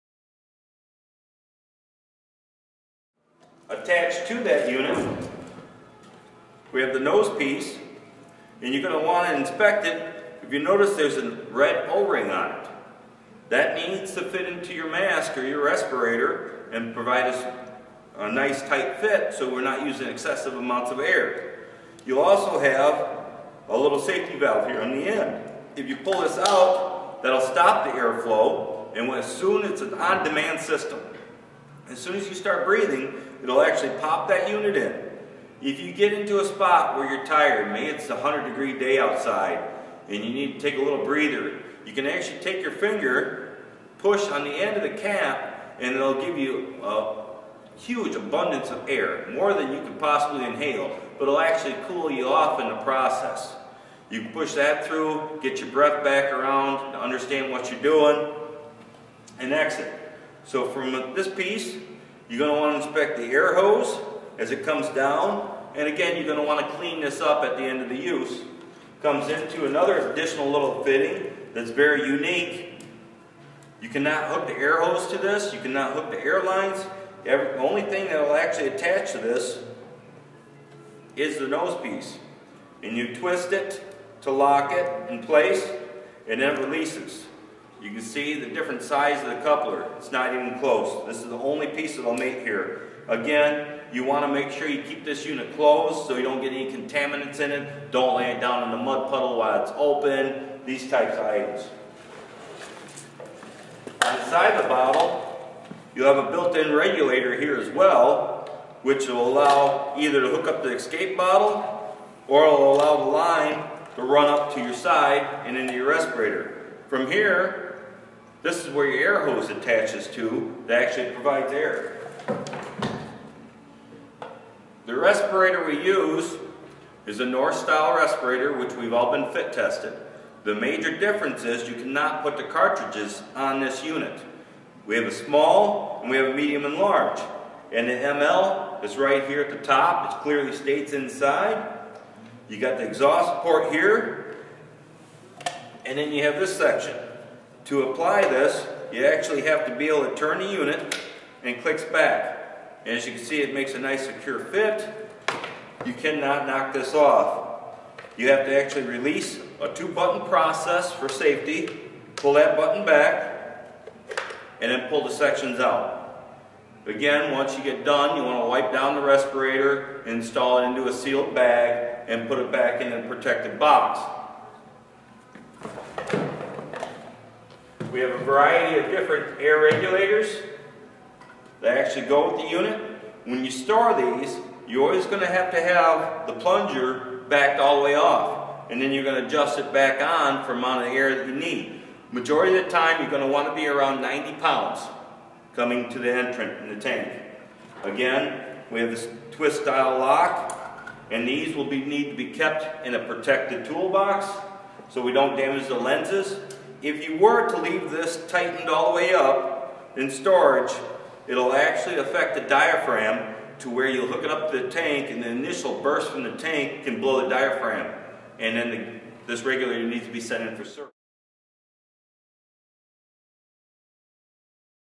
Supplied Air Training
Confined space entry for railcar and tank cleaning might require supplied air.  Tom Wendler explains the nose piece, very important.